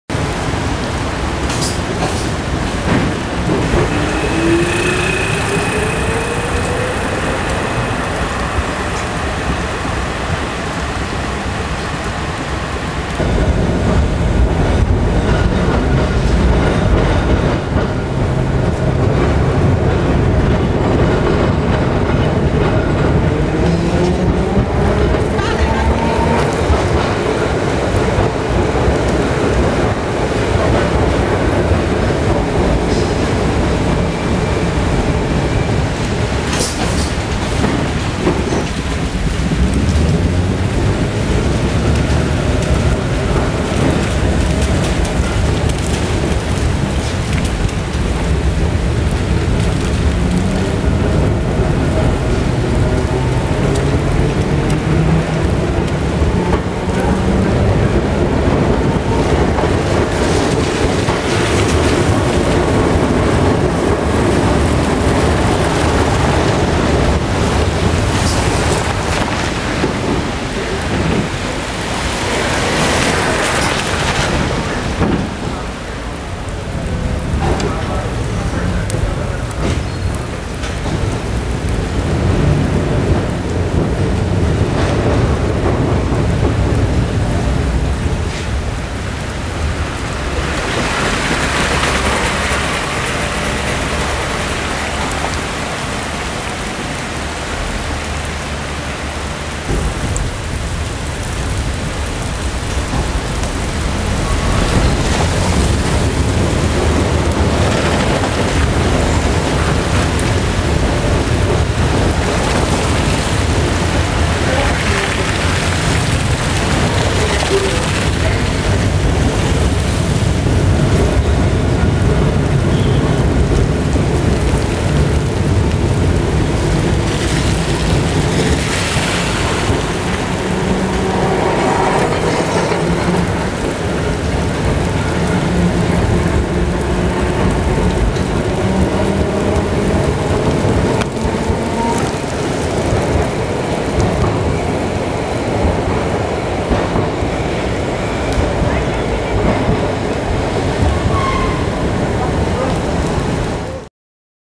Milan Trams Duomo to La Scala
Another very wet ride on a Peter Witt tram 12th September 2012 following Route 1 from around the Duomo and past La Scala Opera House